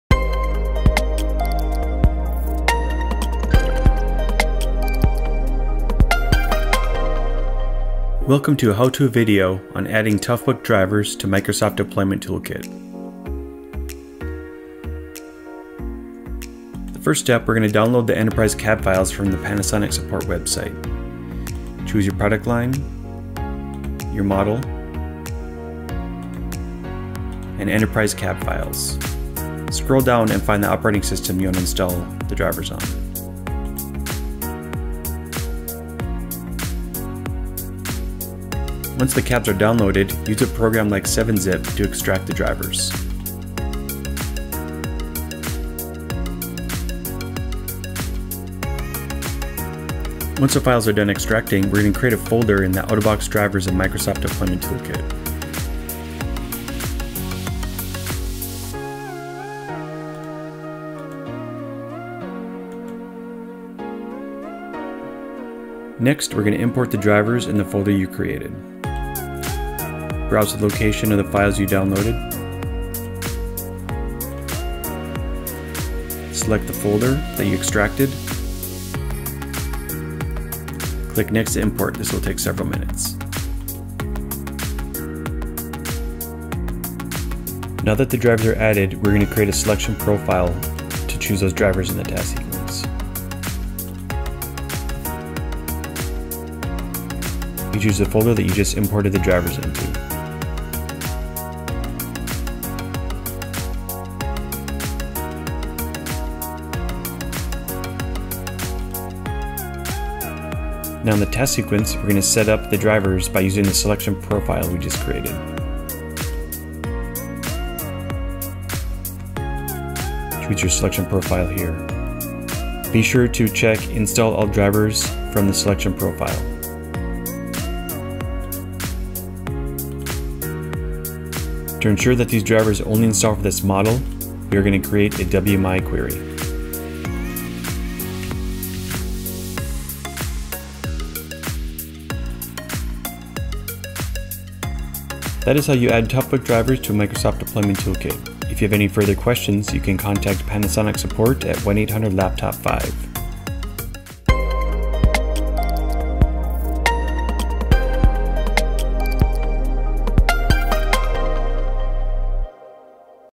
How to Add Panasonic TOUGHBOOK Drivers to Microsoft Deployment Toolkit
A step-by-step tutorial on how to find and add Panasonic TOUGHBOOK drivers to Microsoft Deployment Toolkit